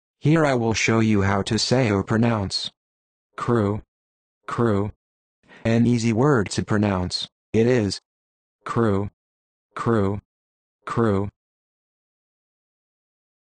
How to Say or Pronounce Crew
This video shows you how to say or pronounce Crew.

How accurate does it say Crew? How would you say Crew?